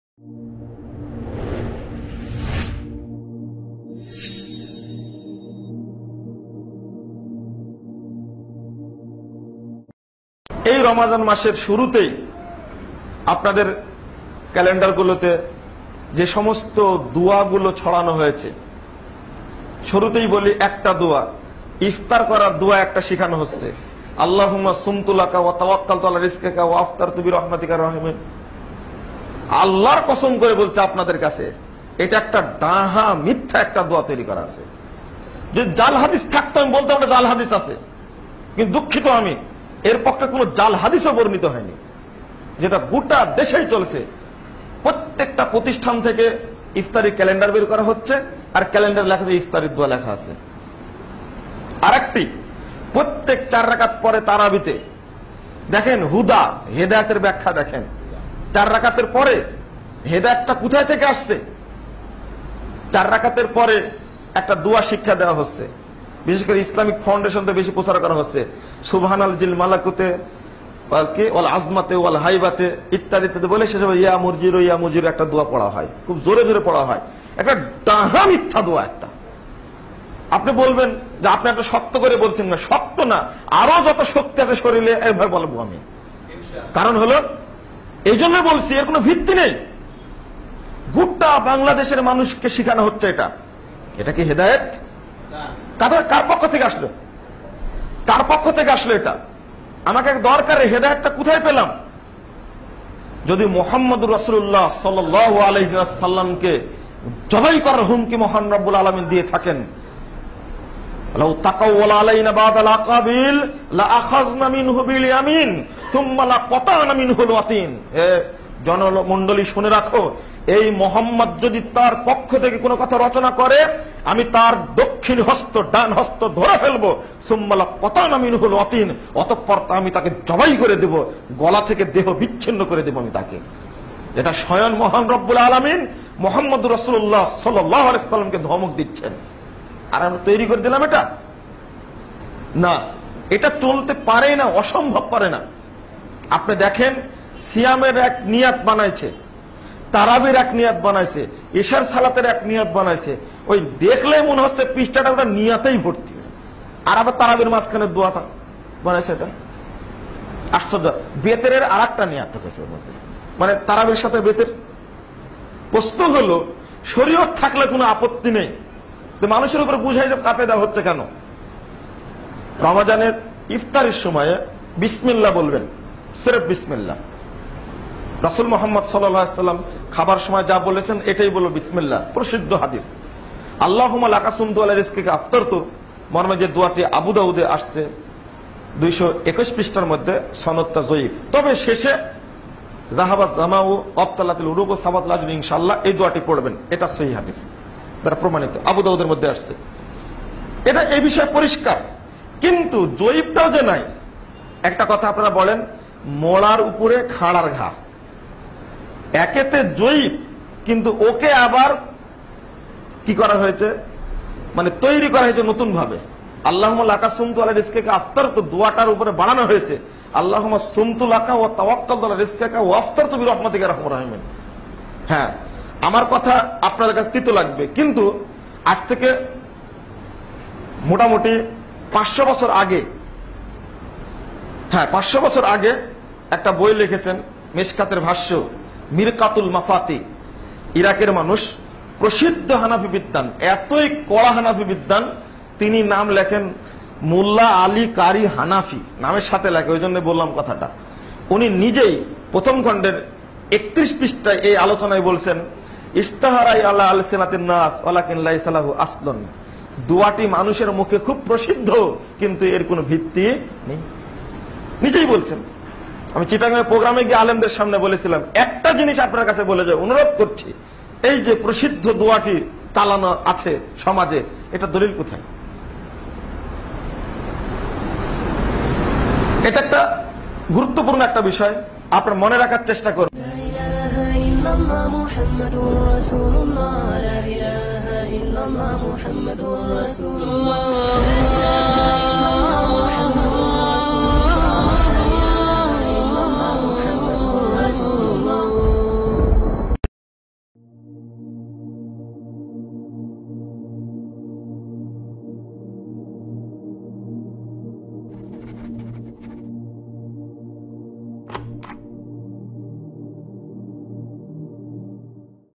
সমাজে প্রচলিত রোযার নিয়ত, ইফতারের দোয়া, তারাবির দোয়া এগুলা কি সহীহ ? শায়খঃ মুজাফফর বিন মহসীন
সমাজে প্রচলিত রোযার নিয়ত,
ইফতারের দোয়া, তারাবির দোয়া এগুলা কি সহীহ ?
শায়খঃ মুজাফফর বিন মহসীন